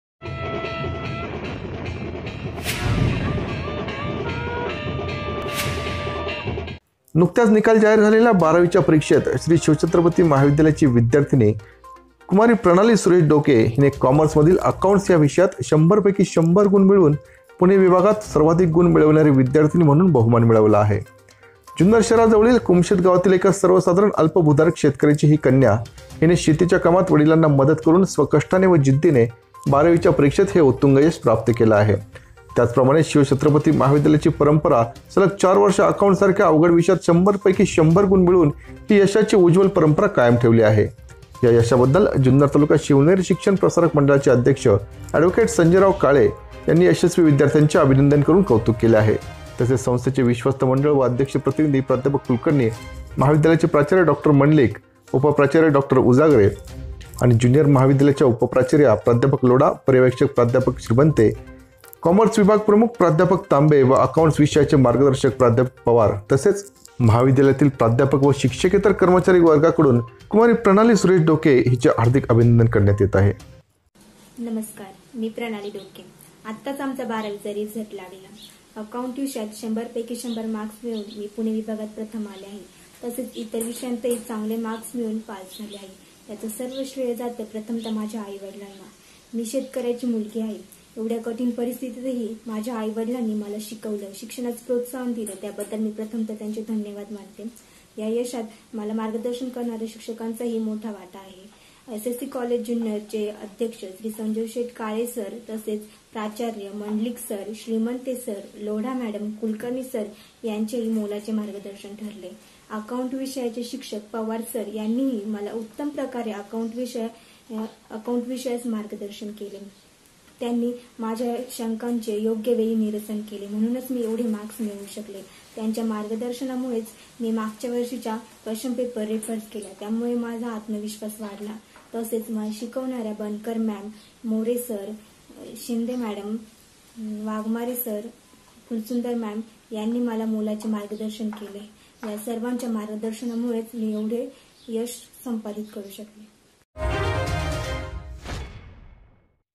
श्रीशिवछत्रपती महाविद्यालयाच्या कु.प्रणाली डोके हिचे उत्तुंग यश,अकाऊंट विषयात मध्ये १०० पैकी १०० गुण
श्री शिवछत्रपती महाविद्यालयाची विद्यार्थिनी व अल्पभूधारक शेतकऱ्याची कन्या कु.प्रणाली सुरेश डोके हिचे उत्तुंग यश,बारावी अकाऊंट विषयात मध्ये १०० पैकी १०० गुण